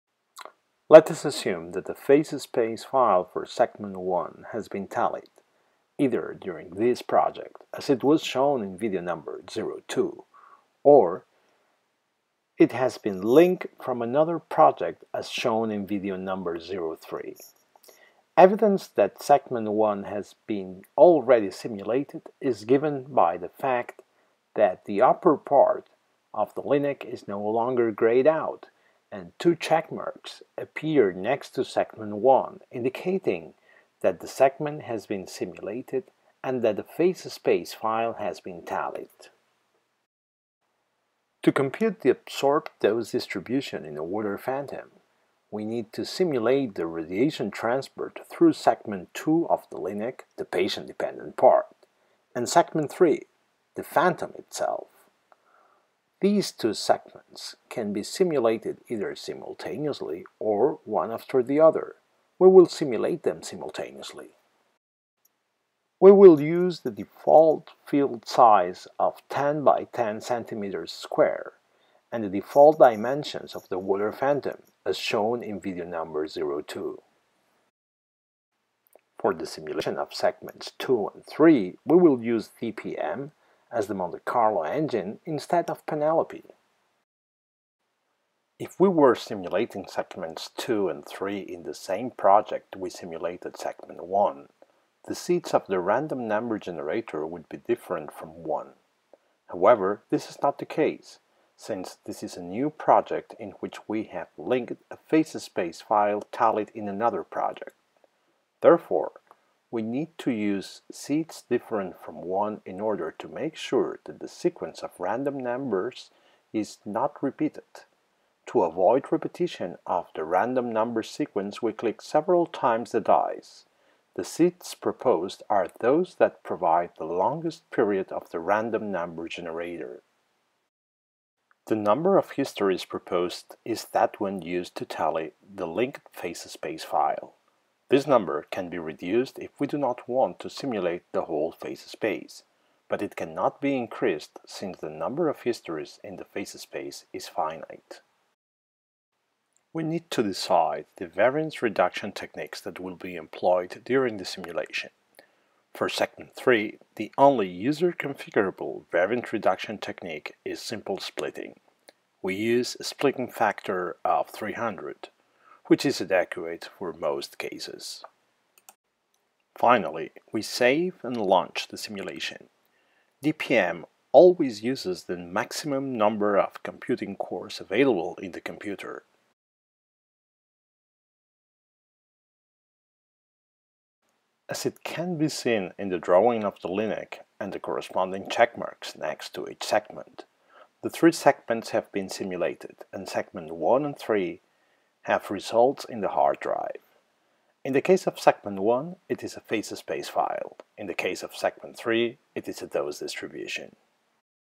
PRIMO tutorial: 04 Dose tallying in water
PRIMO is a Monte Carlo dose verification system for external beam radiotherapy. It uses PENELOPE and DPM as Monte Carlo engines.

The video describes how to tally an absorbed dose distribution in a water phantom.